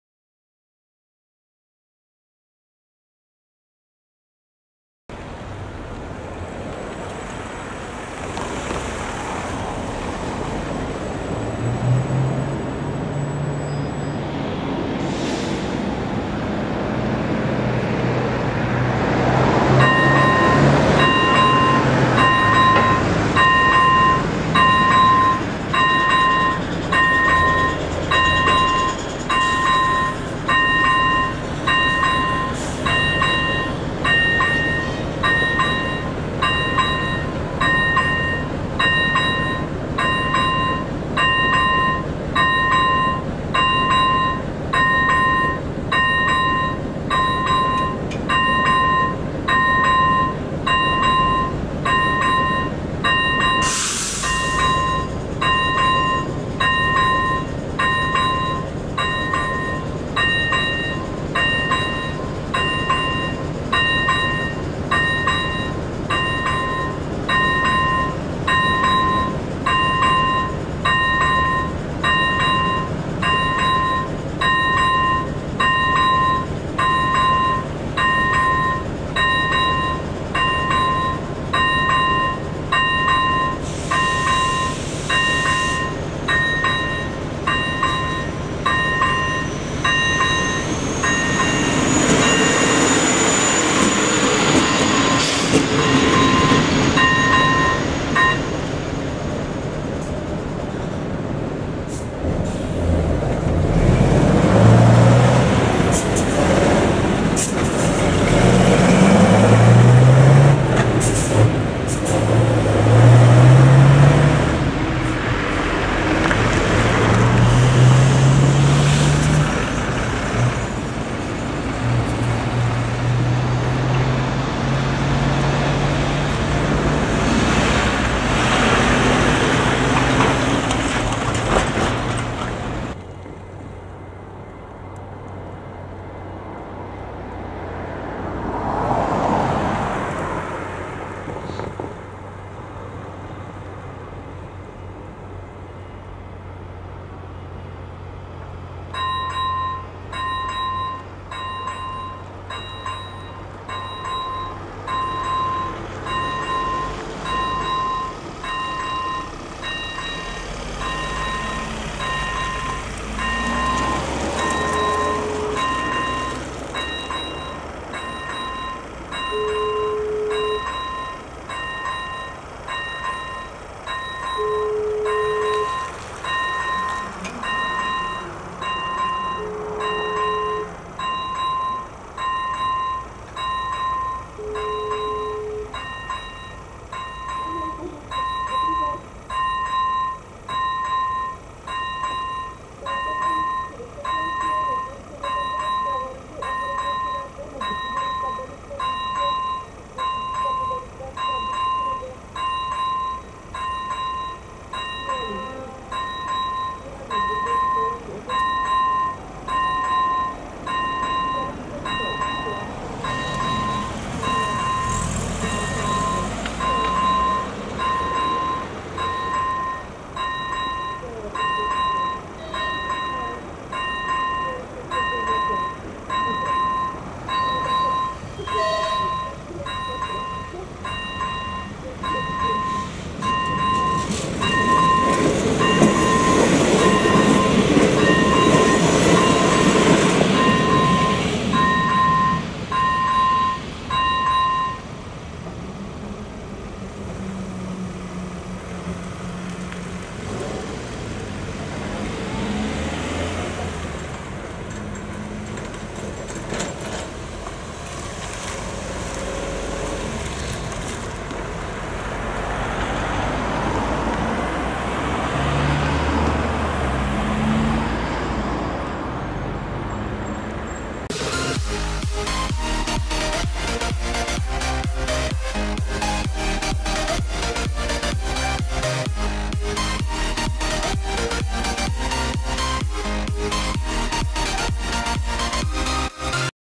Železniční přejezd Vysoké Mýto #2 - 3.3.2022 / Czech railroad crossing
Lokalita: Vysoké Mýto, ul. Vraclavská (u nádraží)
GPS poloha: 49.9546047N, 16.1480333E
Číslo přejezdu: P5162
Trať: SZCZ 018 (Choceň - Litomyšl)
Úsek: Vysoké Mýto - Vysoké Mýto město
Výstražníky: AŽD 97
Zvonek: ZV-01

Vlaky ve videu:
1) Os 15044, ČD (Choceň - Vysoké Mýto město) 9:58
2) Os 15007, ČD (Litomyšl - Choceň) 10:09

Spolupráce: Veselka91 - Railway, Jakub - Czech railroad crossing
Datum: 3.3.2022
Natočeno na kameru: Panasonic HC-V380